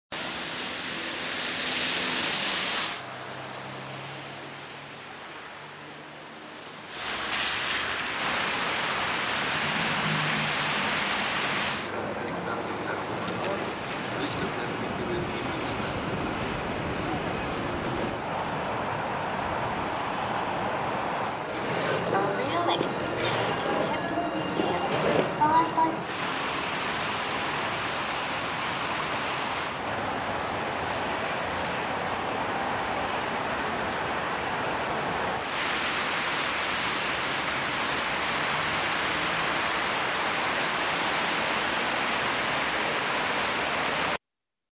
Moscow on the first day of Spring
01 March 2013
Moscow welcomes spring in a distinct Russian style as heavy snow blankets the capital city.